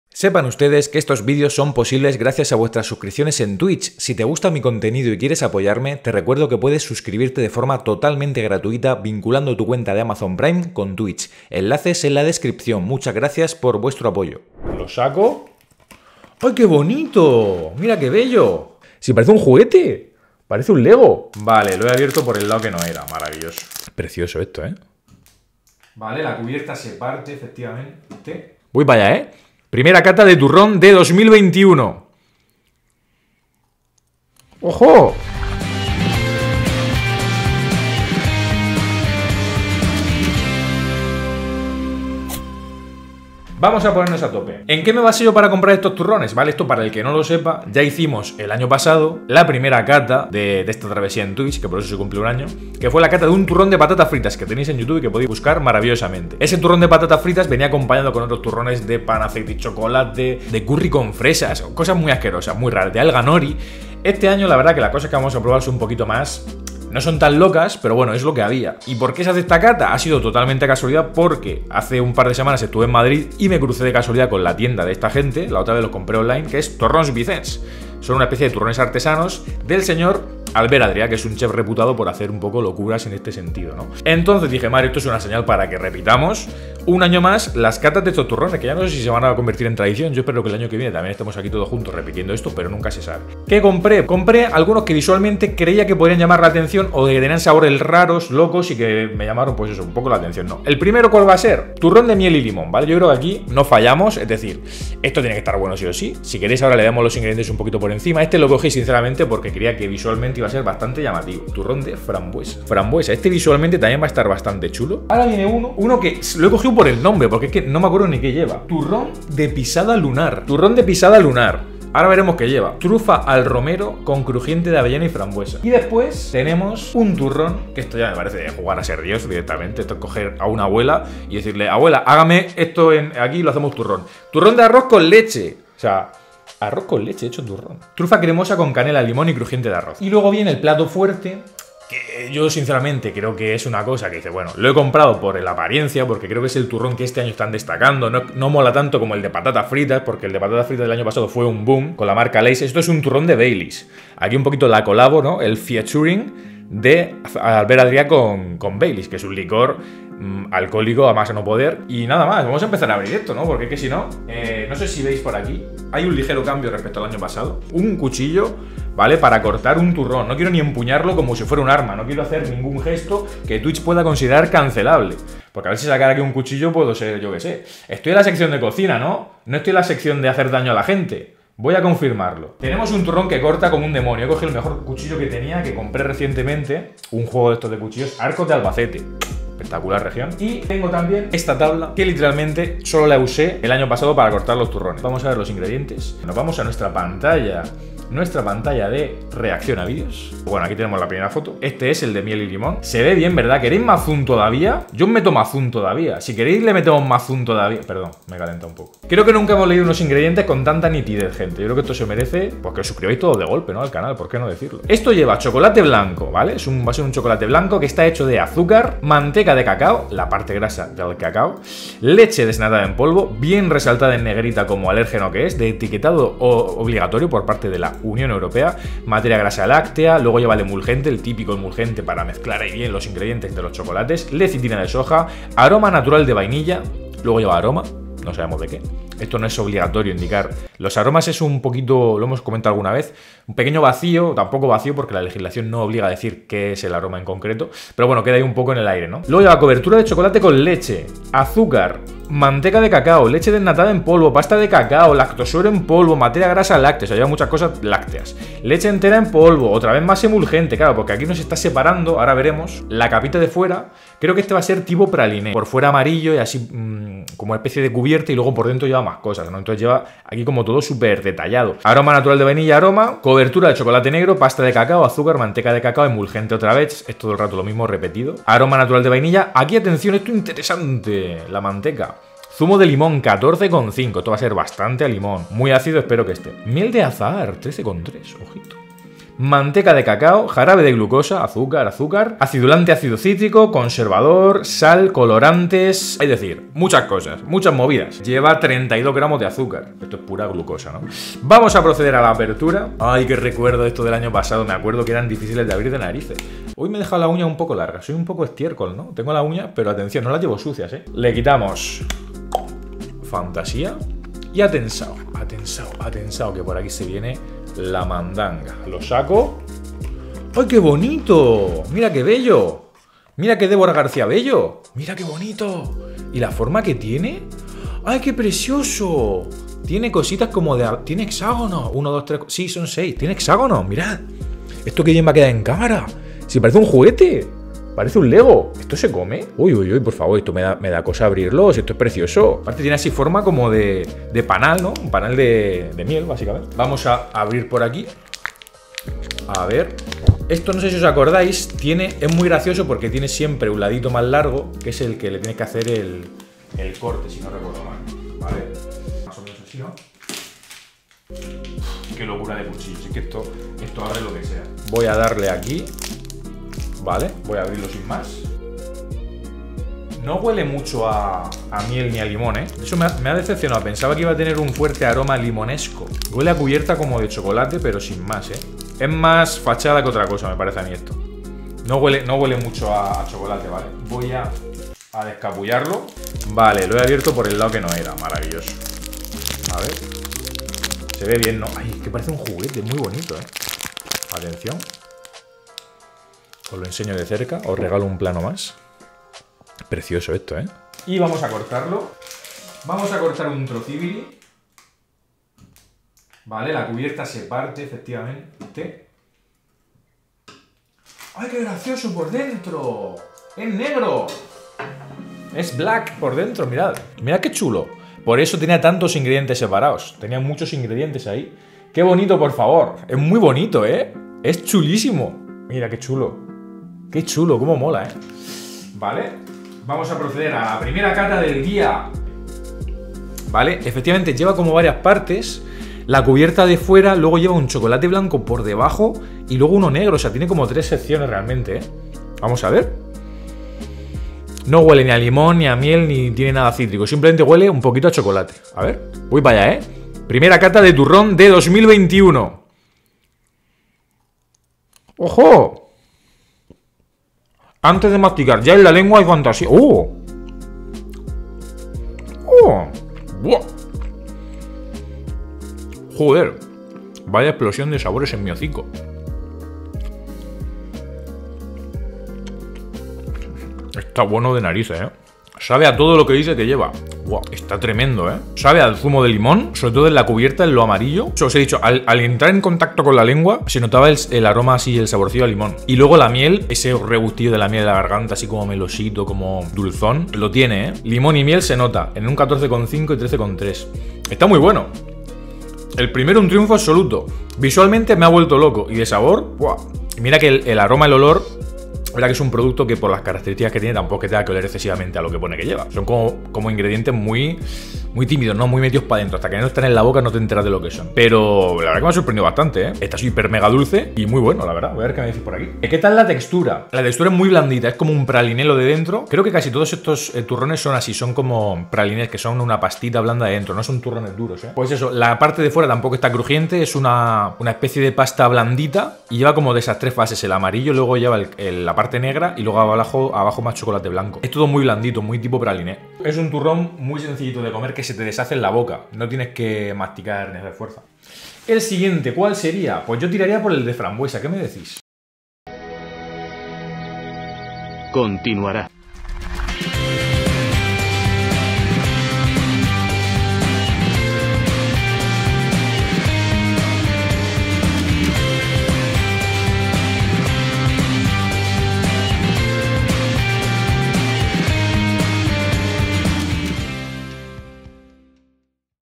¡ESTE TURRÓN PARECE UN LEGO! Probando TURRÓN MIEL y LIMÓN by Albert Adrià
Volvemos a hincarle el diente a los turrones extravagantes del chef Albert Adrià y Torrons Vicens por Navidad. Empezamos con este turrón de miel y limón que tiene una apariencia de lo más rocambolesca.

► Este clip ha sido extraído de mi canal de Twitch ¡Pásate por allí para disfrutar de más contenido alimentario en directo!